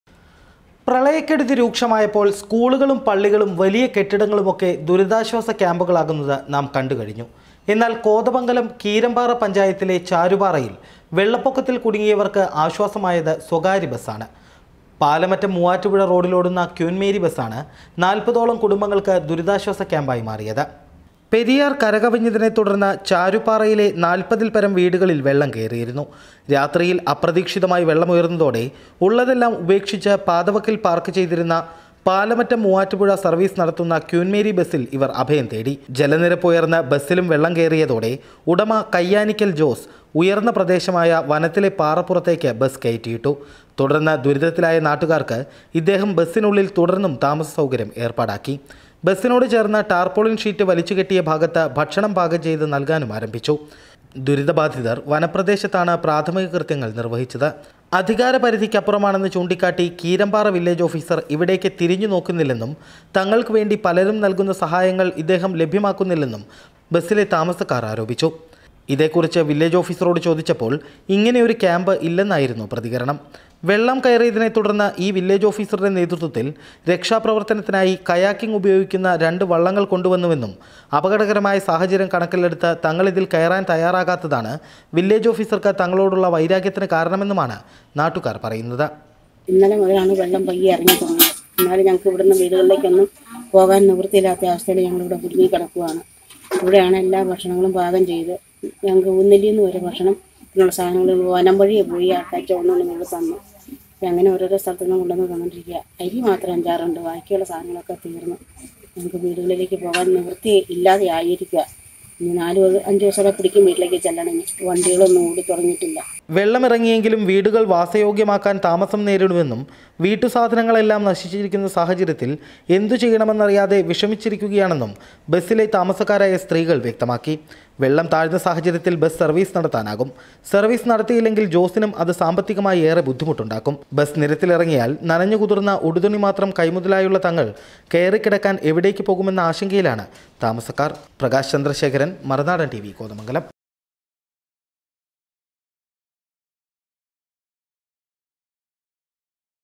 കീരംപാറയില്‍ സ്വകാര്യ ബസ് ദുരിതാശ്വാസ ക്യാമ്പായ കഥ-queenmary bus
കീരംപാറയില്‍ സ്വകാര്യ ബസ് ദുരിതാശ്വാസ ക്യാമ്പായ കഥ